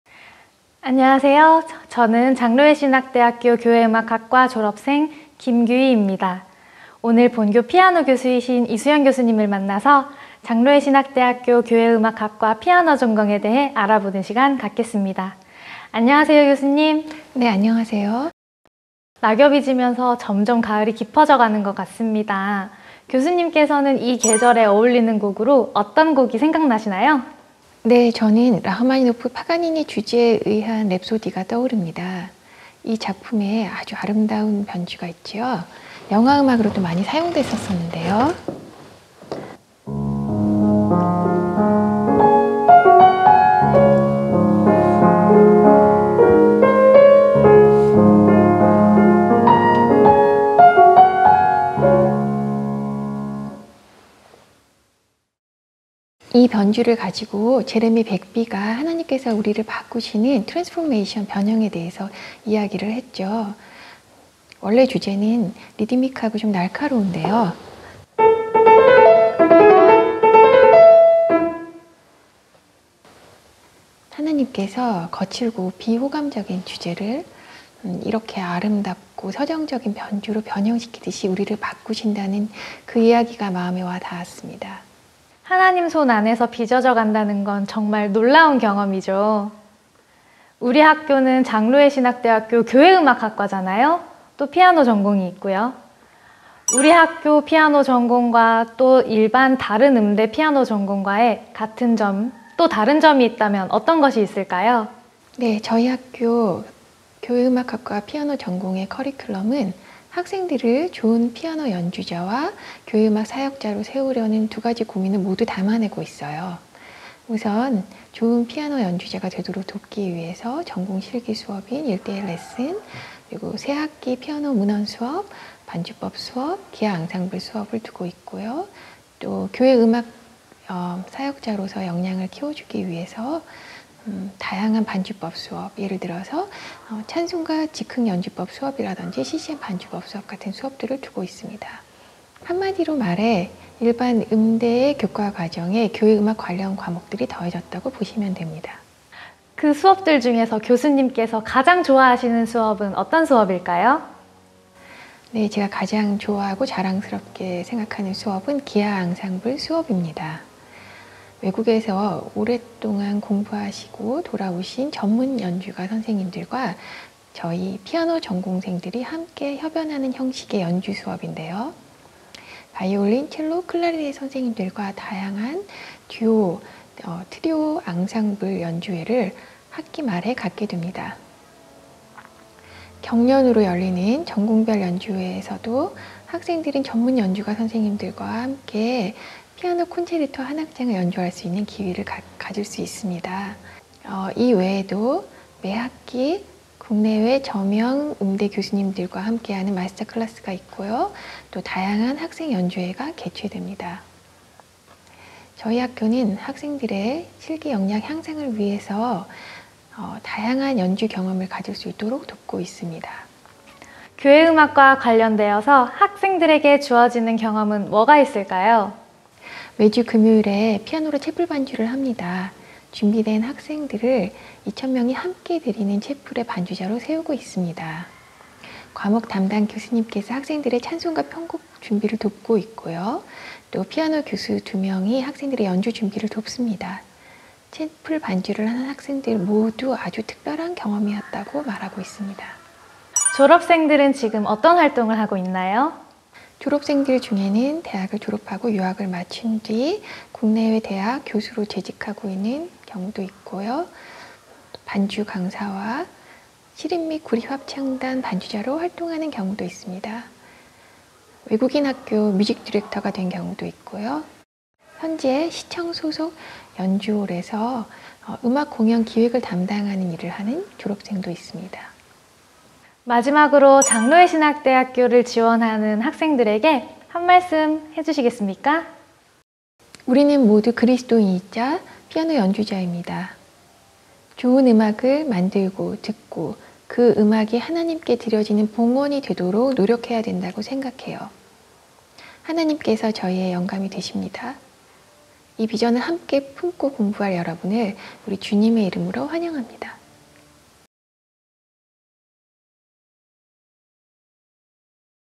[입시] 2022 교회음악학과 피아노전공 소개영상 | 장로회신학대학교
장로회신학대학교 교회음악학과 피아노 전공을 소개합니다!